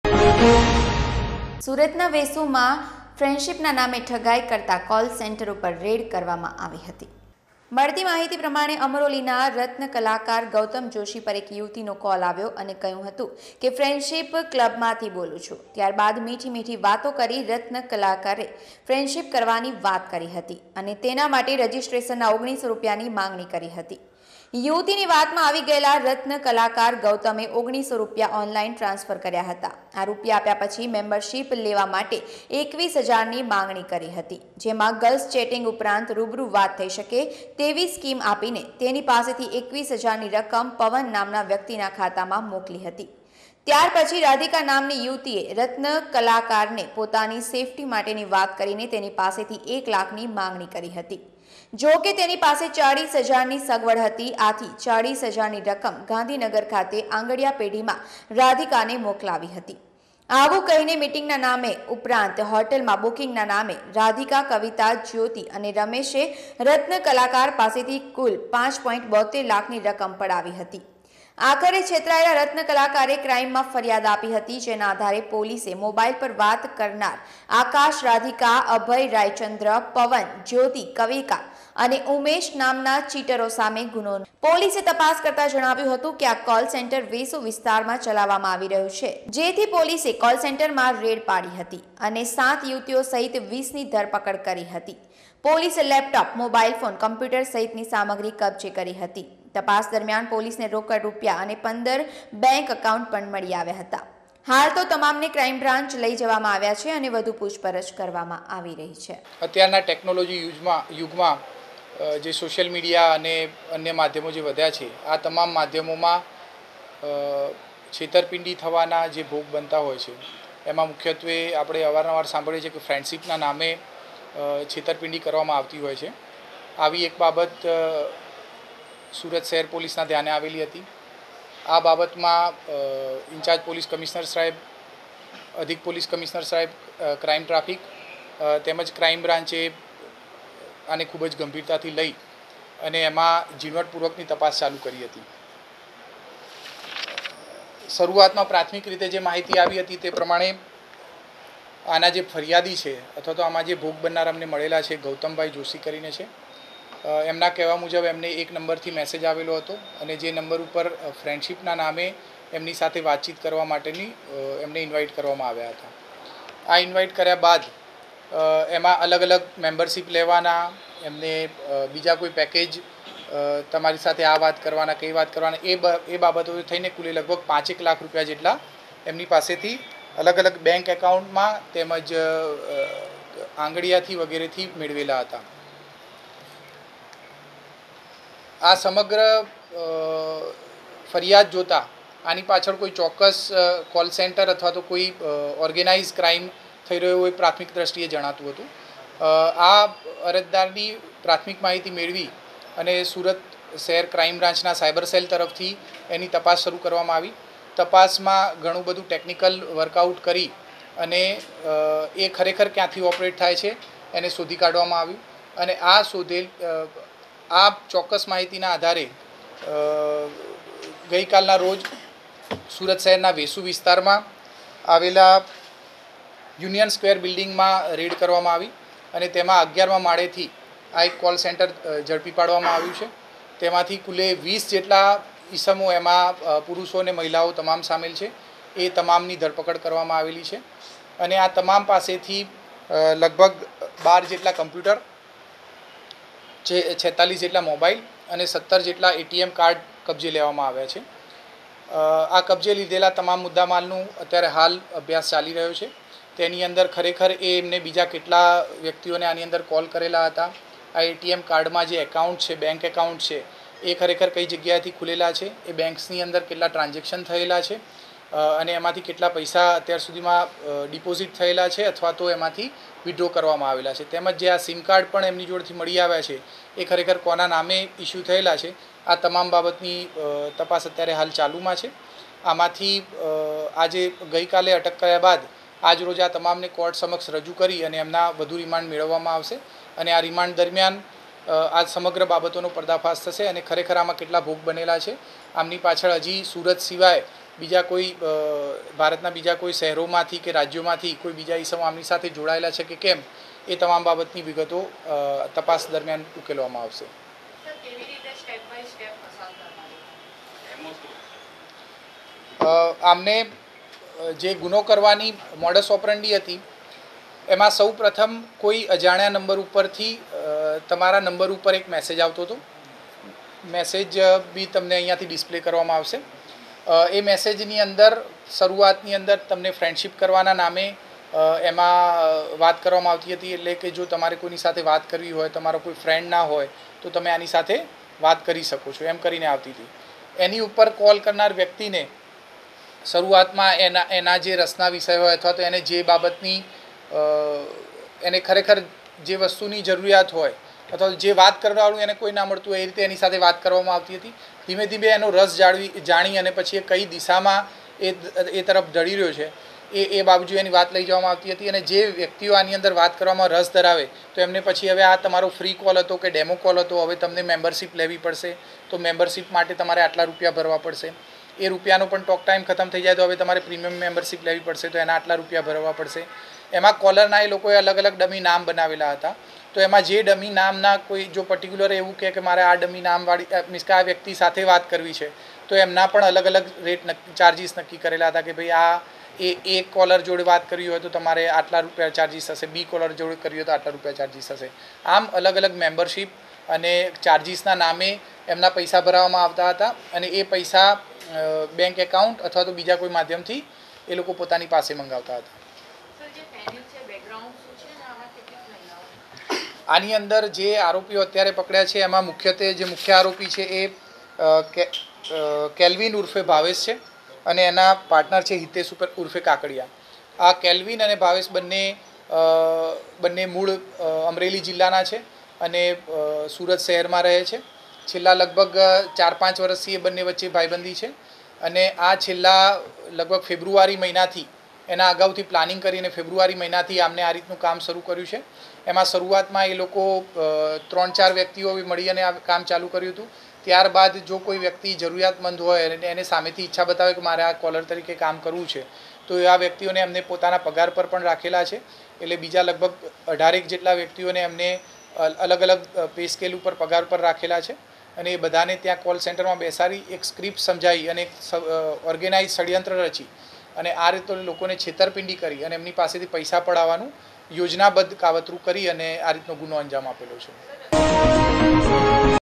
LOKMAT NEWS VADODARA
વેસુમાં ફ્રેન્ડશીપના નામે ઠગાઈ કરતા કોલસેન્ટર પર પોલીસની રેડ, 7 યુવતીઓ સહિત 20ની ધરપકડ